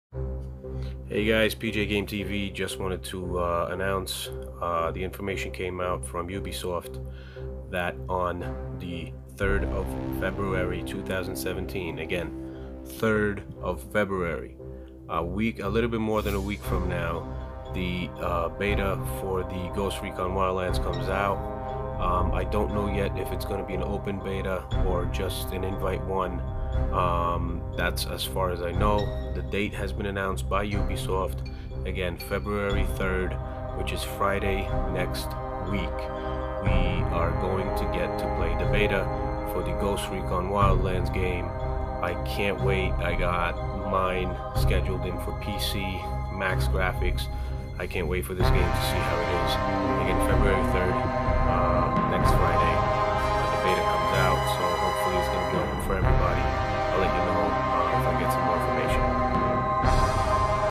Ghost Recon Wildland BETA release date
This is official info from Ubisoft on the release date for Ghost Recon Wildlands Beta.